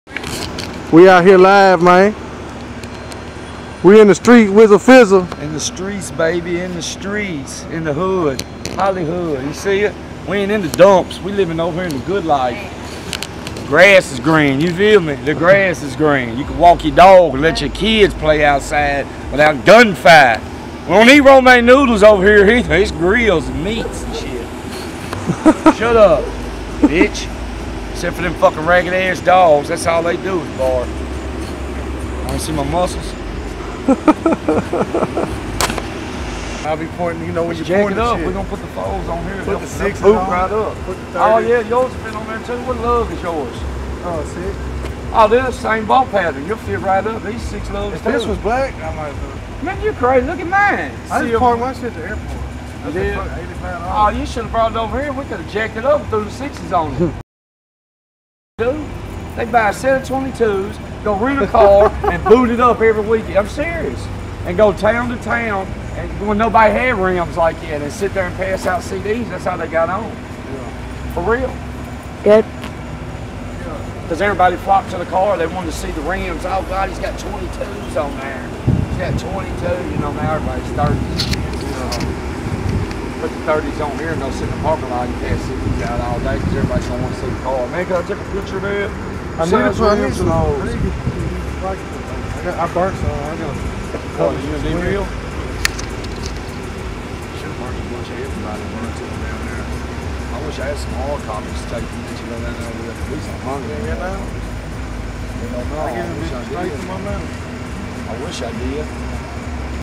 Nashville Rapper Worm 2019 The Last 2 Minutes Of Footage I Got Before He Was Locked Up
I was on my way to Atlanta GA and stopped by to see wizzle fizzle aka worm before I hit the interstate. Its the last time I ever saw him before he went to jail. Free worm!!